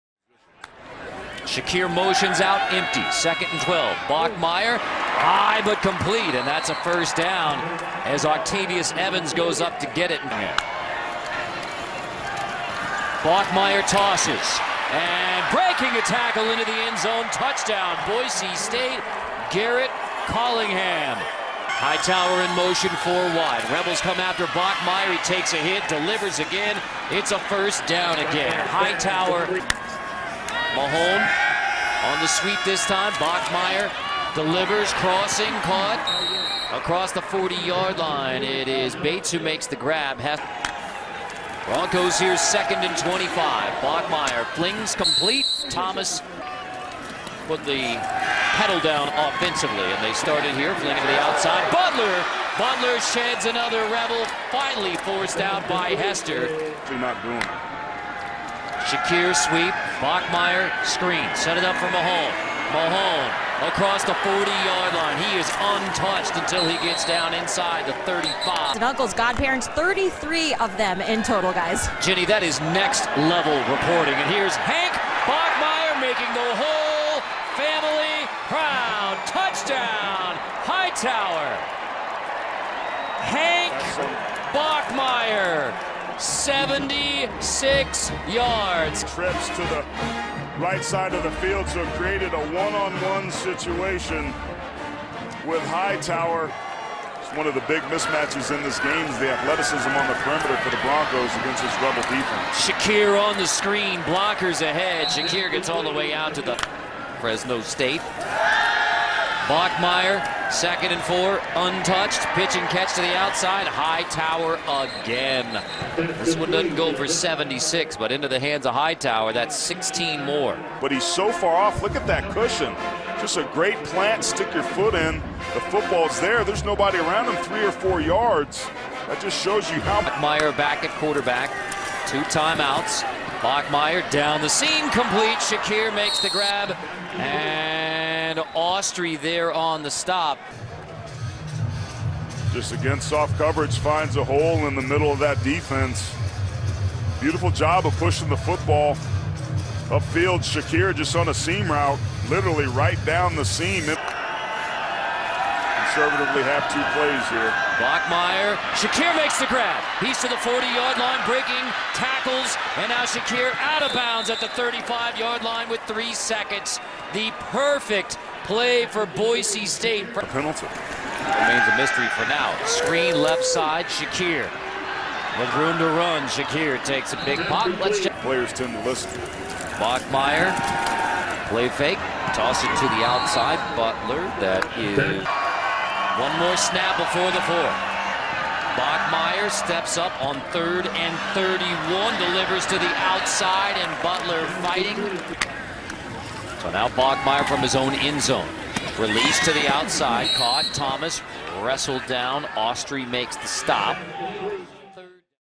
Hank Bachmeier Highlights vs UNLV | NCAA
Boise State QB Hank Bachmeier went 19-30 for 299 yards and 2 touchdowns.Comment suggestions for future videos and enjoy!